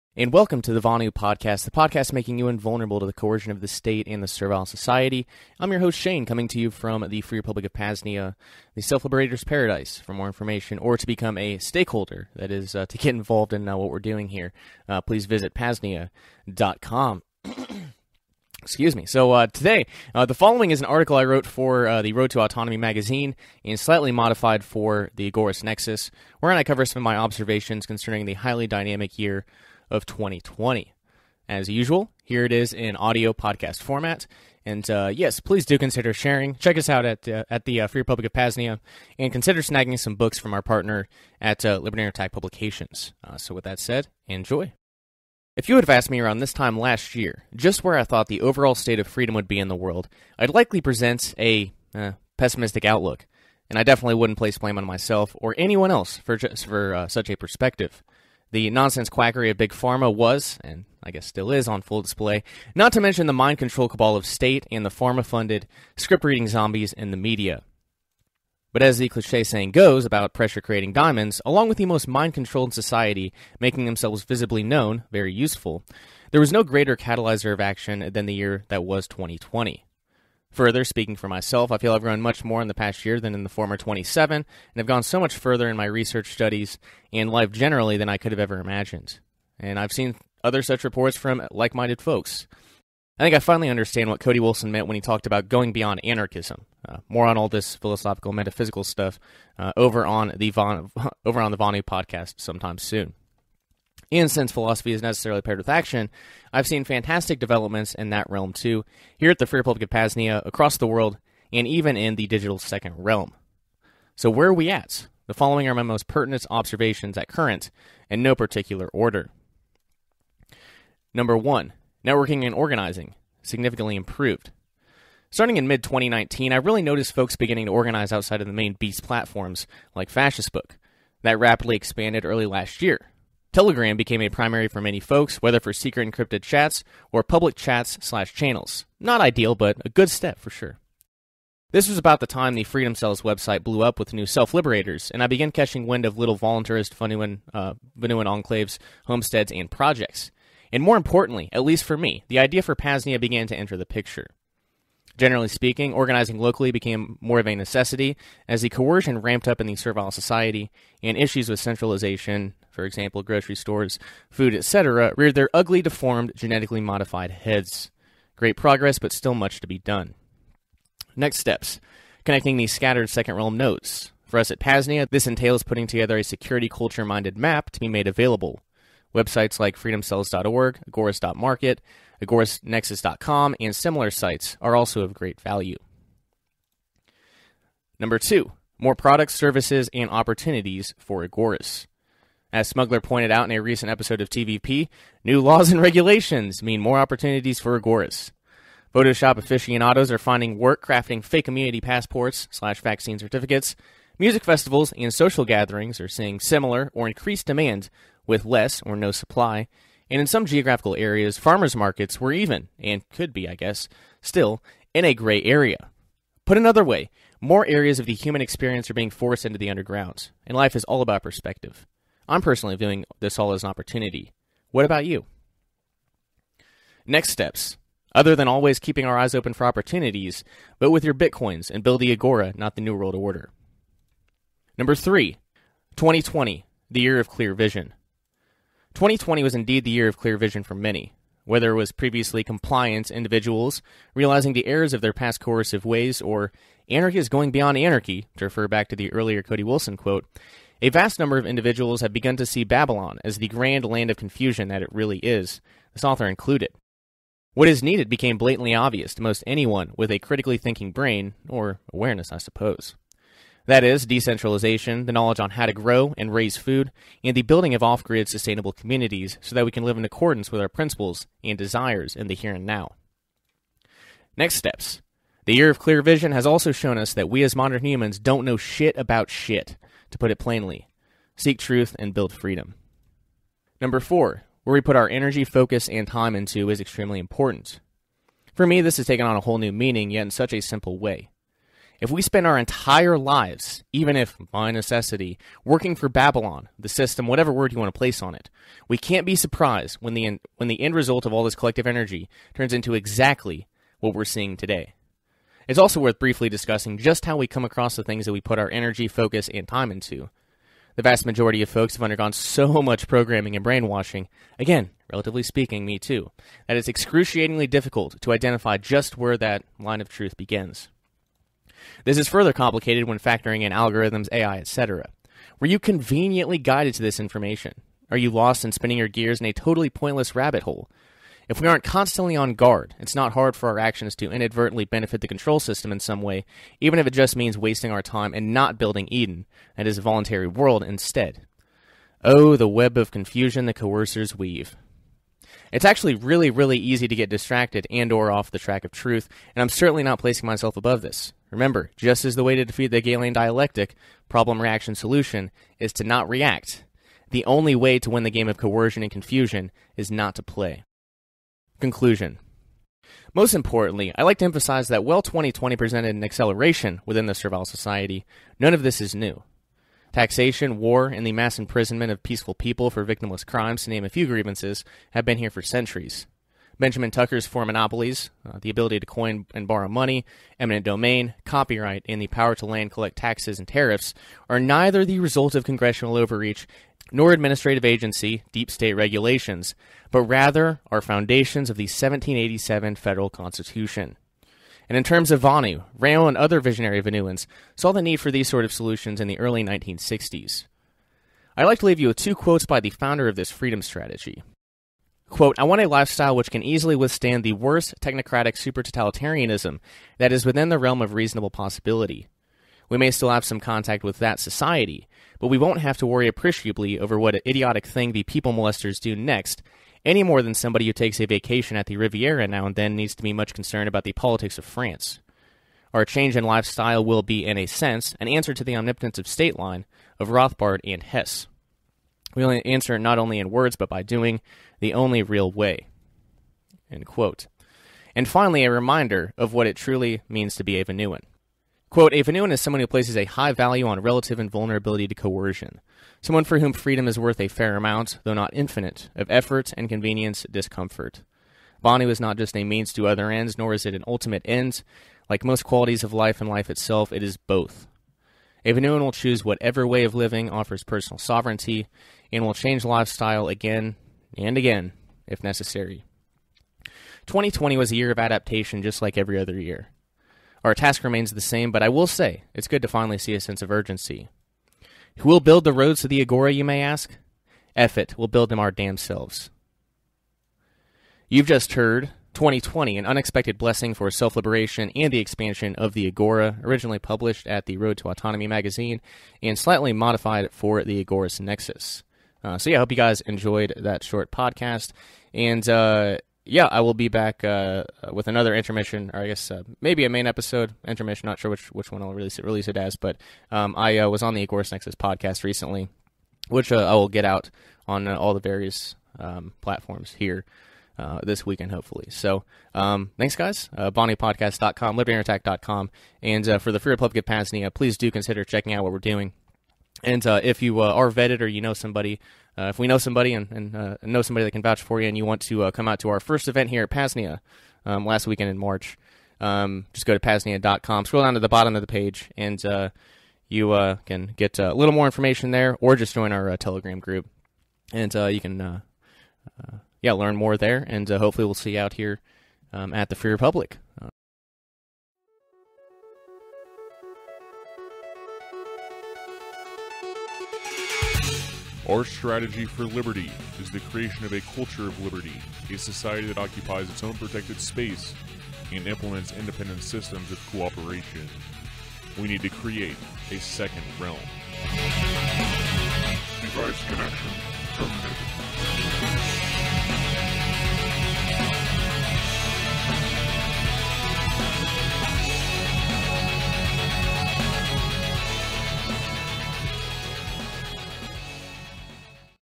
TVP #108: [2020] An Unexpected Blessing For Self-Liberation
The following is an article I wrote for the Road to Autonomy Magazine, and slightly modified for the Agorist Nexus, wherein I cover some of my observations concerning the highly dynamic year of 2020.

As usual, here it is in video/podcast format...please enjoy.

[LIBERTY UNDER ATTACK PUBLICATIONS : Books, Discounted Bundles, Audiobooks, & Privacy Tools!]